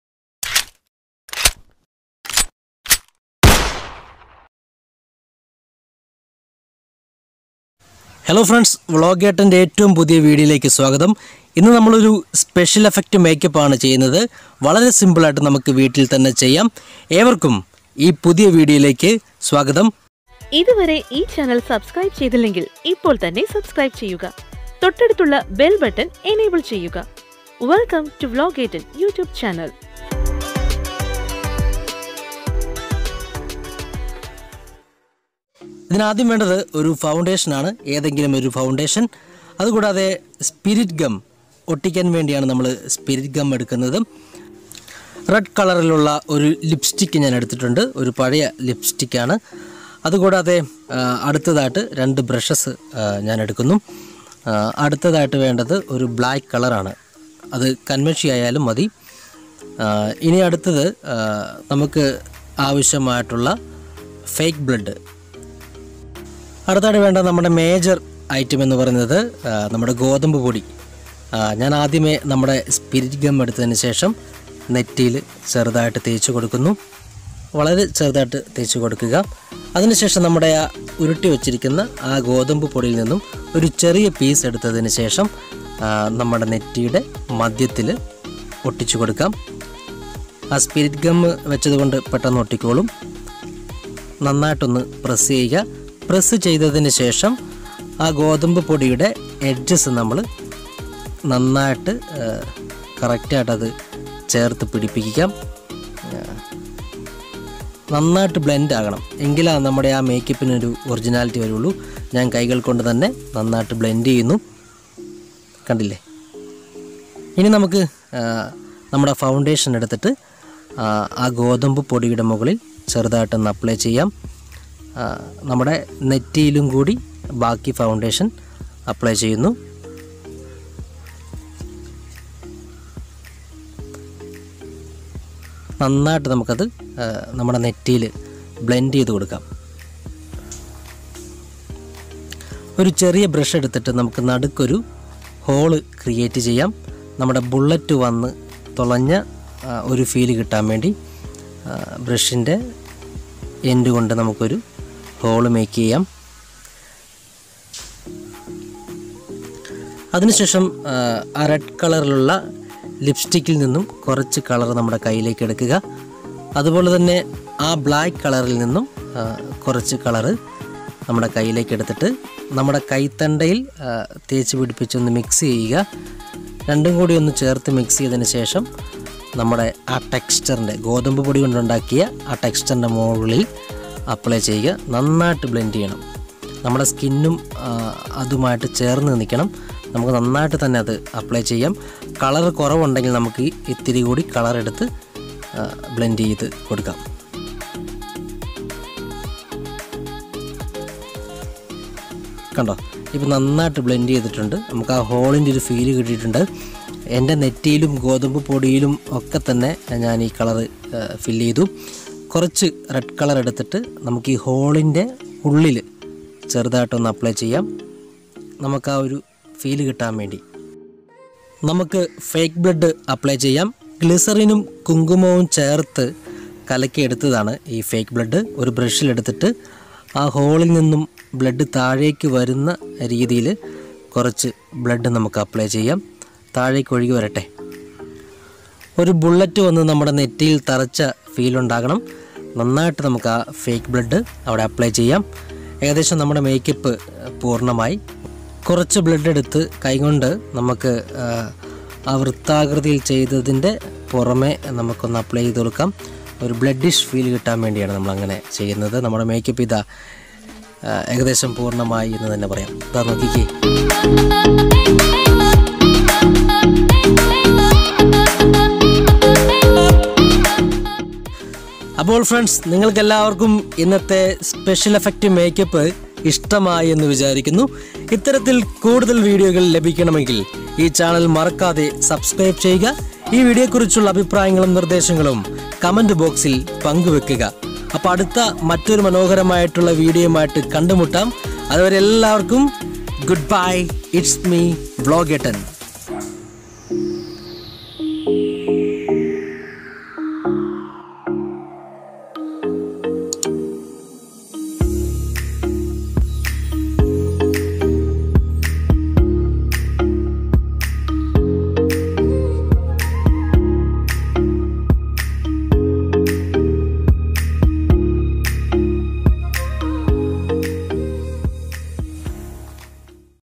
ഗോതമ്പുപൊടി കൊണ്ടൊരു മാജിക് | Special Effect Makeup Tutorial Malayalam
Create a classic gun shot wound in a few short minutes. This halloween makeup is very easy to create and can be applied anywhere on your body.
My Gears📱
Camera | Open Camera
Video Editing&DI | Kine master
Audio Editing | Voice Over
Design | Malayalam Image Editor | Makron | Snapseed

അങ്ങനെ അങ്ങു പോയാലോ✌
ഇതങ്ങു ഞെക്കി പൊട്ടിച്ചേക്കു🔔